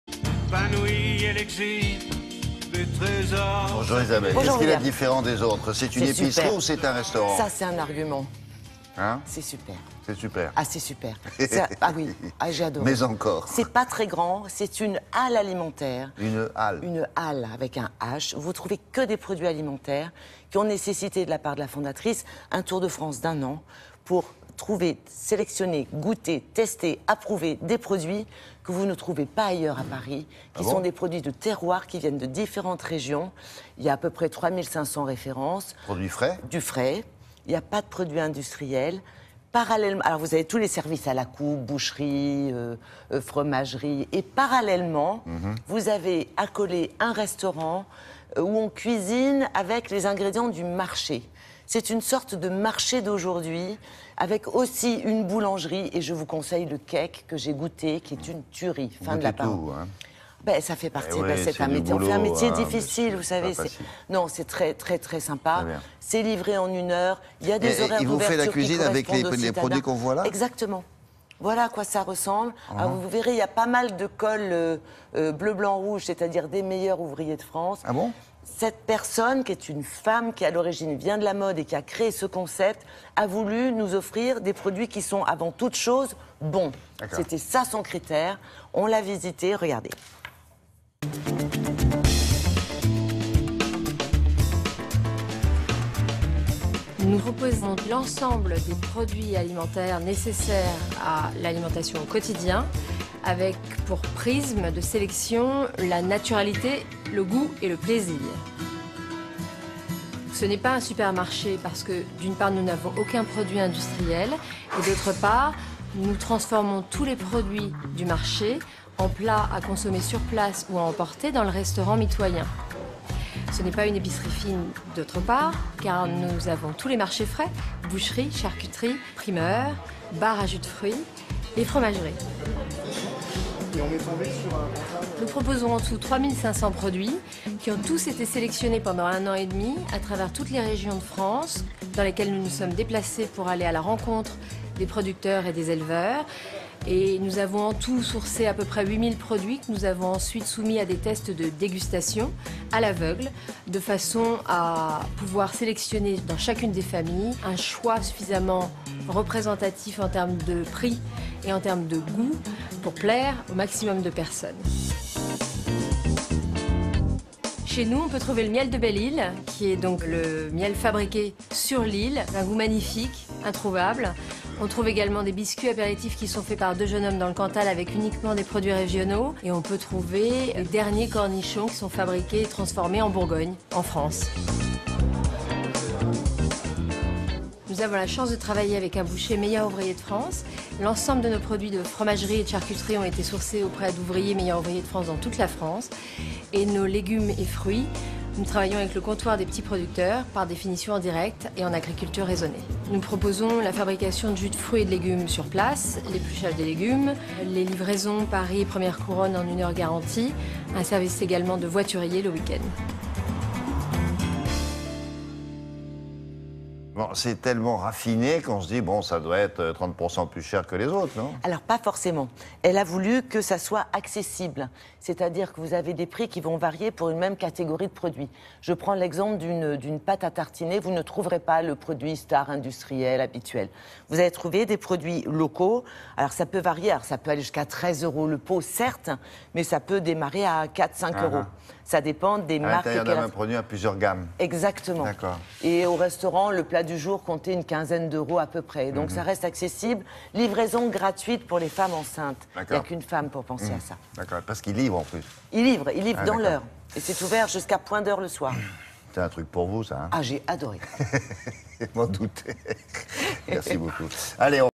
Conso-Maison Plisson
Isabelle nous fait visiter la Maison Plisson, une alimentation générale installée depuis peu à Paris.